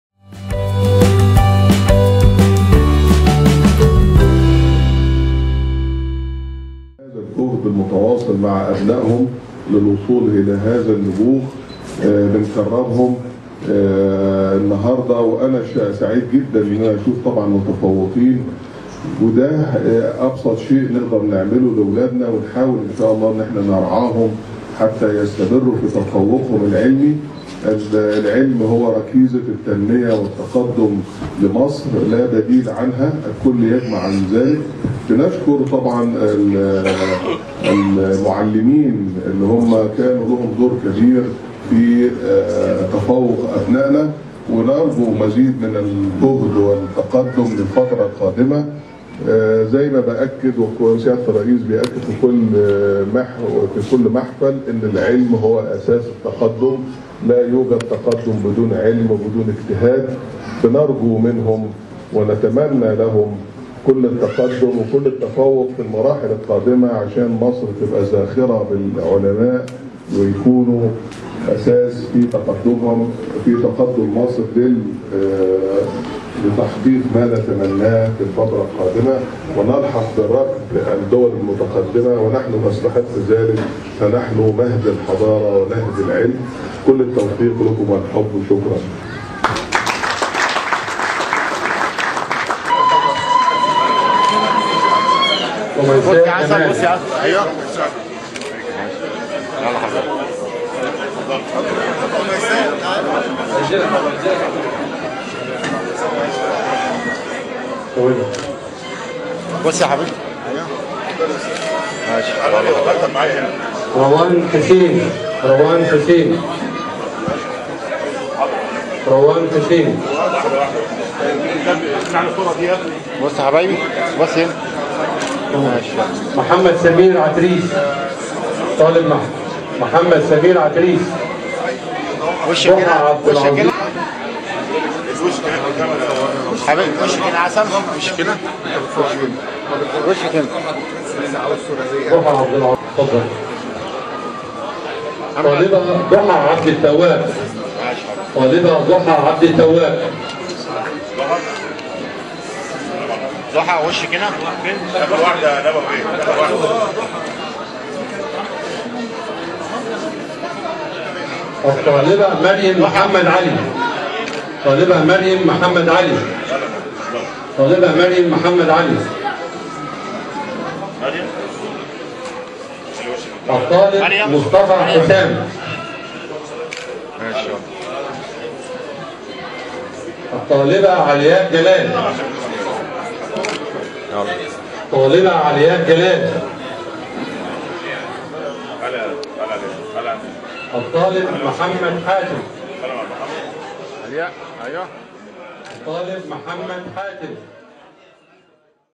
فيديو : محافظ بني سويف يكًرم أوائل الابتدائية والاعدادية على مستوى المحافظة‎
بحضور المهندس «شريف حبيب» محافظ بني سويف وقيادات وزارة التربية والتعليم أختفلت مديرية التربية والتعليم بالمحافظة بتكريم العديد من أوائل الشهادات الابتدائية والاعدادية.